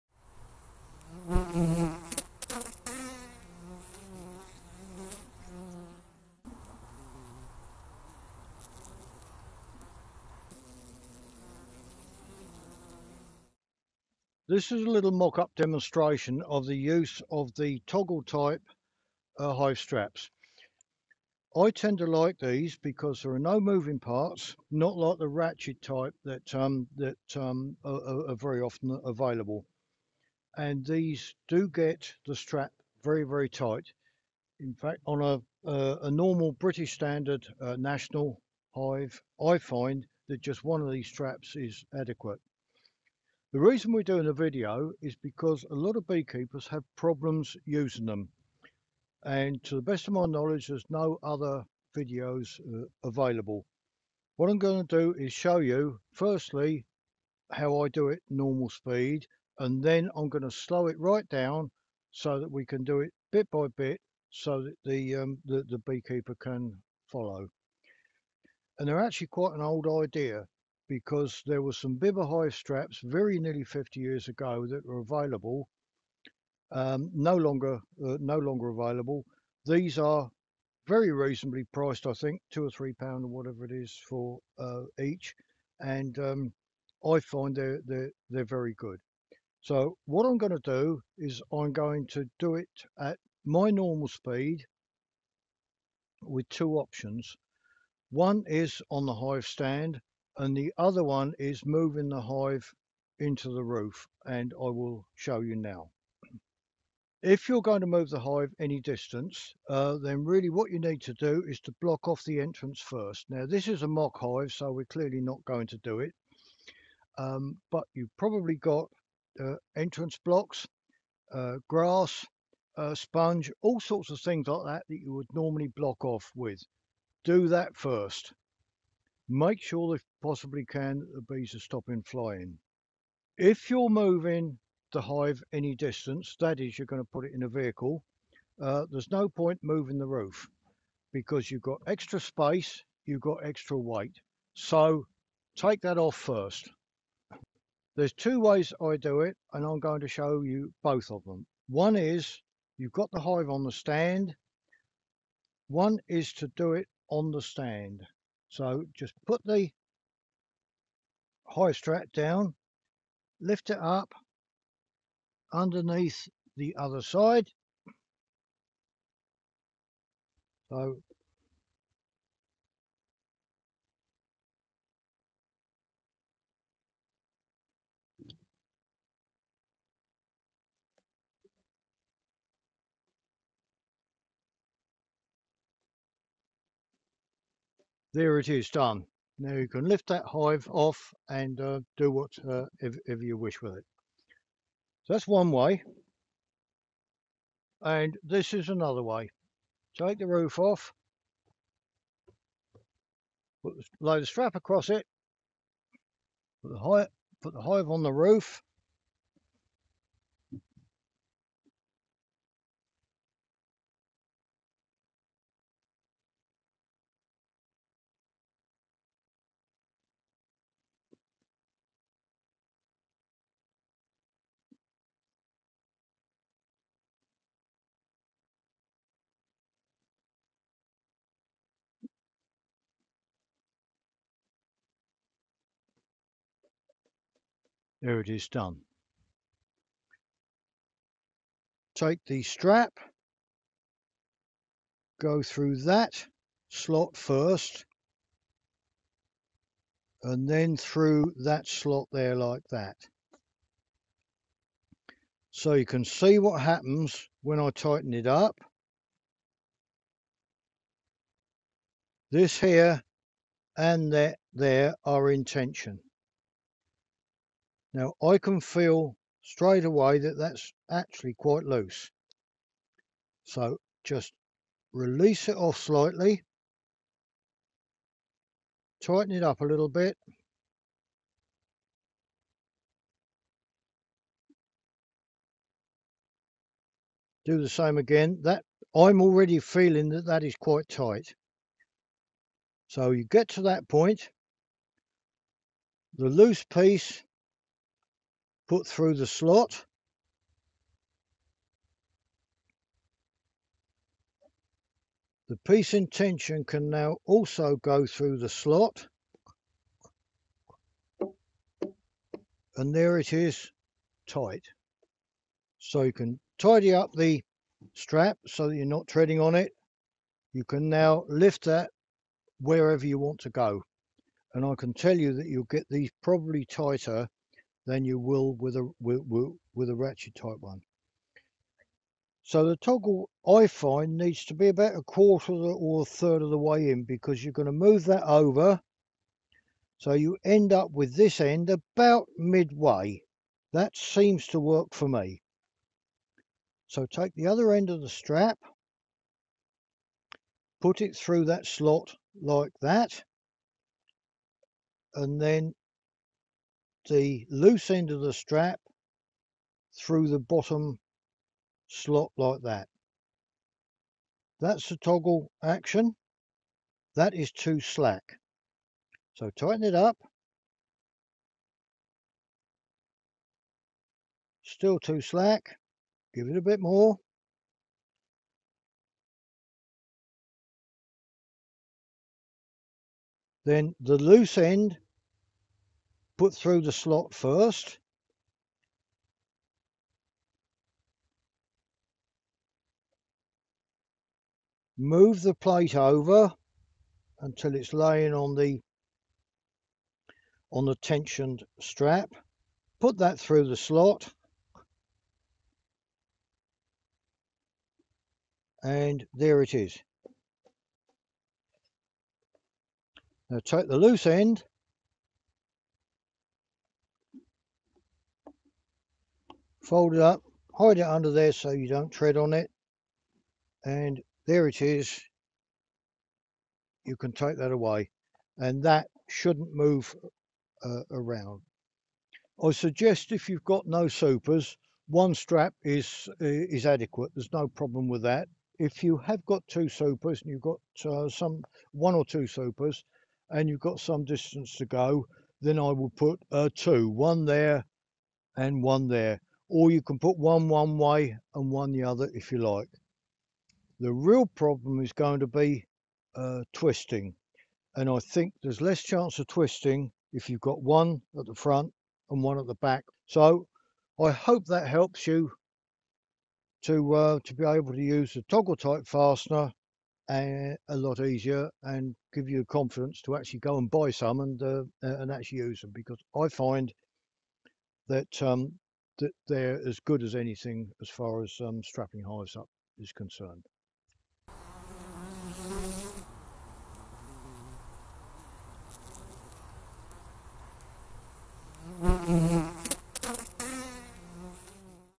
Using a Toggle Hive Strap
The toggle hive strap is often looks daunting to someone who hasn't used one before. Roger shows us how to strap a hive up using this ingenious little device that looks very similar to an old BIBBA product.

To avoid annoyance we don’t enable adverts on our videos.